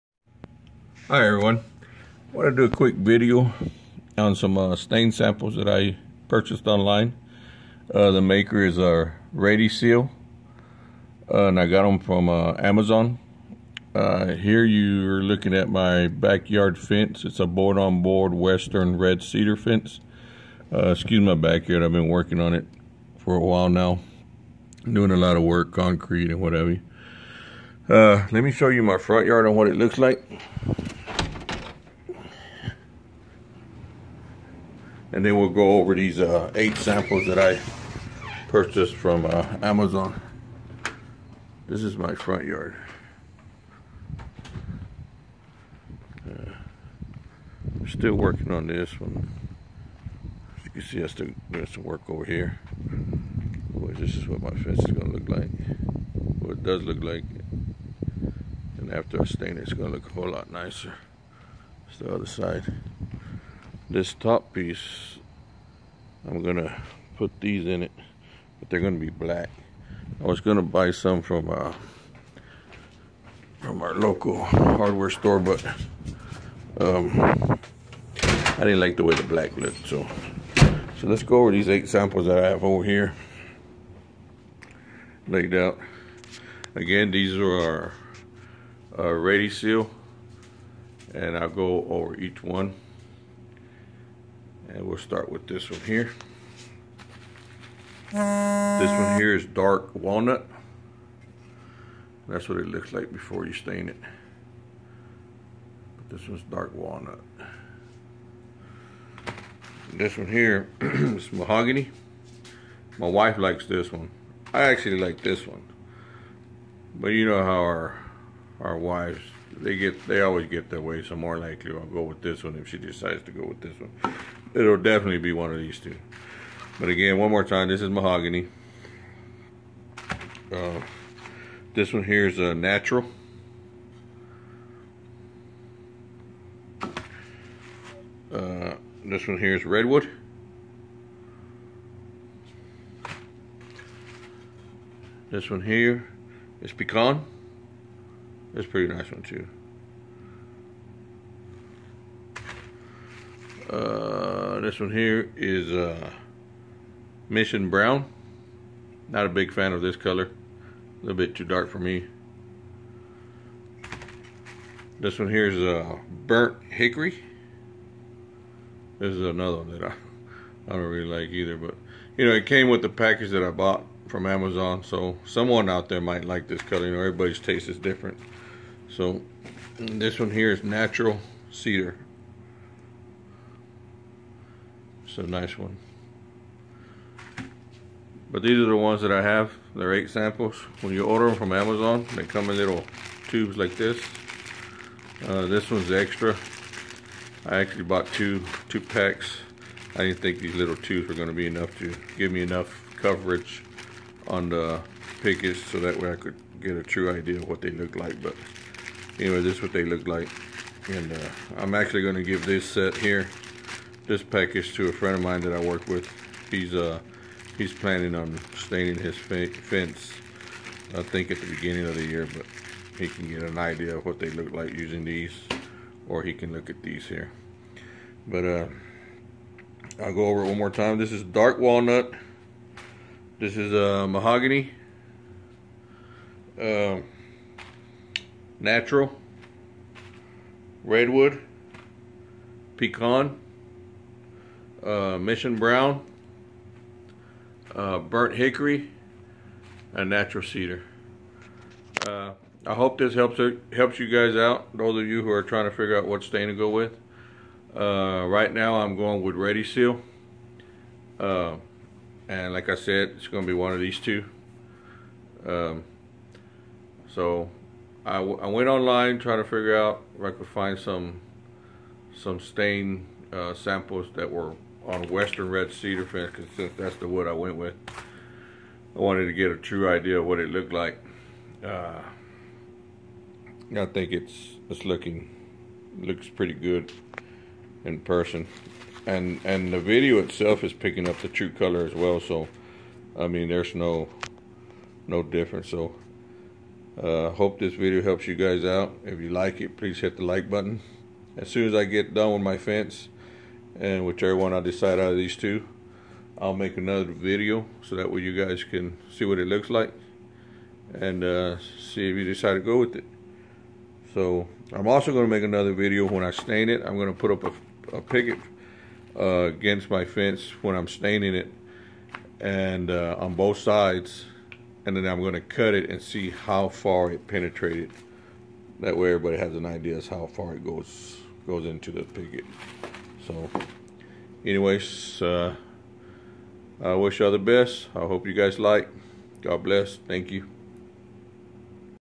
Ready Seal Stain Samples on Western Red Cedar wood
8 - Samples of stain on Western Red Cedar wood.

Ready Seal 8 Color Sample Pack